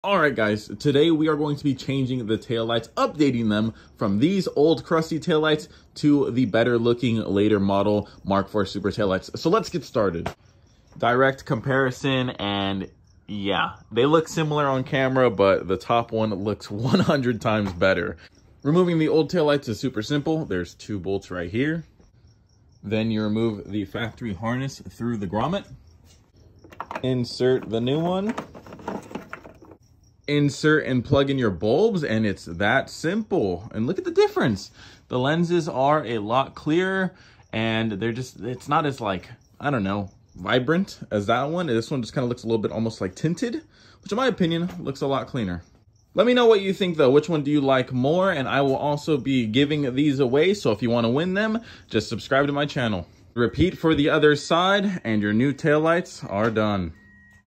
Mk4 Supra Gets New Taillights!
Tossing on a pair of later model mk4 Supra taillights to update the look of the rear end